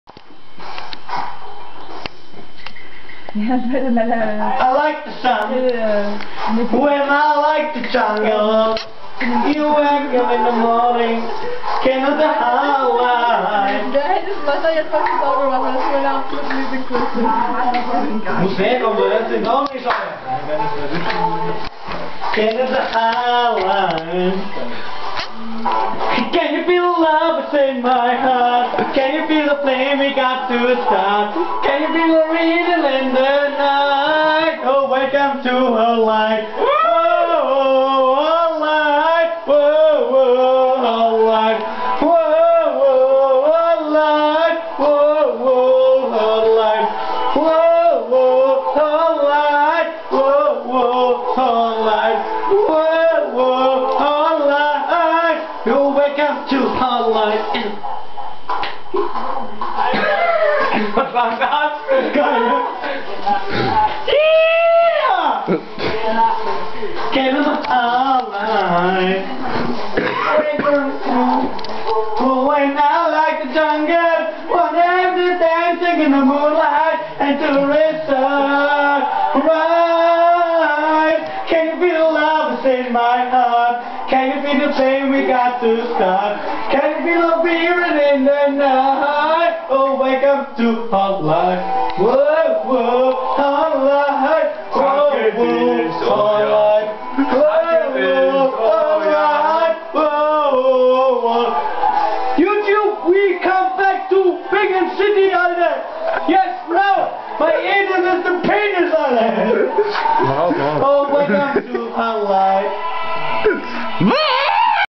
junger Mann singt vom harten Leben
da folgende video is anch starkem Alkoholeinflusses auf eine hausparty entstanden...
viel spaß